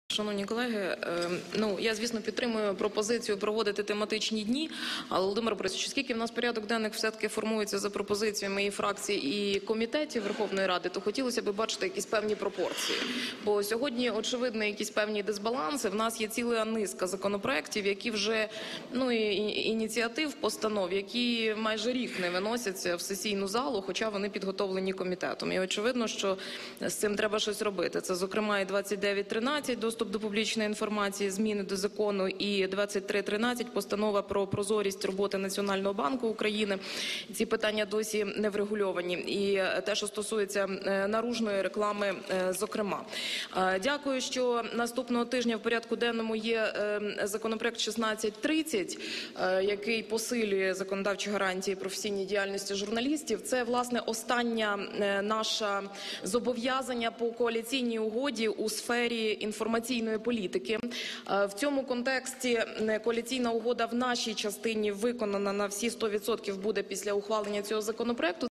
Коаліційна угода у сфері інформаційної політики виконана майже на 100%
Для повного виконання коаліційної угоди у сфері інформаційної політики залишилось ухвалити лише один законопроект - №1630, щодо посилення законодавчих гарантій професійної діяльності журналістів.

Про це заявила голова Комітету ВР з питань свободи слова та інформаційної політики, заступник голови депутатської фракції «Народний фронт» Вікторія Сюмар сьогодні під час засідання Погоджувальної ради.

«Це, власне, останнє наше зобов’язання по коаліційній угоді у сфері інформаційної політики. В цьому контексті Коаліційна угода в нашій частині буде виконана на всі 100% після ухвалення цього законопроекту», - зазначила вона.

У той же час заступник голови депутатської фракції «Народний фронт» закликала керівництво Верховної Ради відновити тематичний баланс законопроектів, які розглядаються у сесійній залі.

«Я, звісно, підтримую пропозицію проводити тематичні дні у парламенті, але, оскільки у нас порядок денний формується за пропозиціями фракцій і комітетів Верховної Ради, то хотілось би бачити якісь пропорції. Сьогодні очевидний певний дисбаланс. У нас на сьогодні є ціла низка ініціатив і постанов, які вже майже рік не виносяться у сесійну залу, хоча вони підготовлені комітетом», - зазначила вона.

Зокрема, за словами Вікторії Сюмар, мова йде про законопроекти щодо доступу до публічної інформації, щодо зовнішньої реклами, а також про постанову щодо прозорості роботи Національного банку України.

«Ці питання досі не врегульовані», - додала вона.

Приєднуйся!